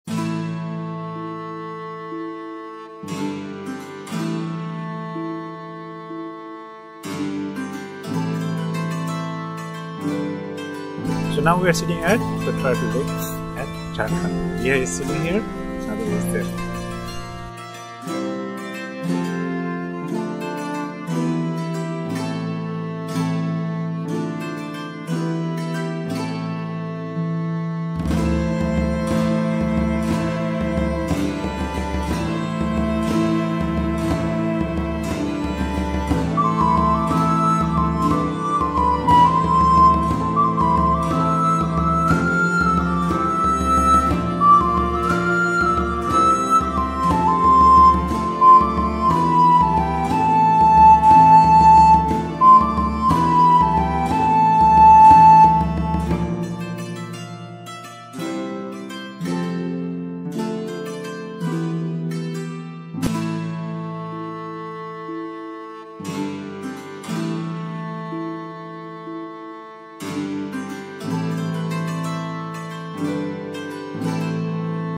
Jharkhand Diaries | Teaser
This is a teaser video on our long weekend road-trip to Jharkhand in the last week of February in the year 2020.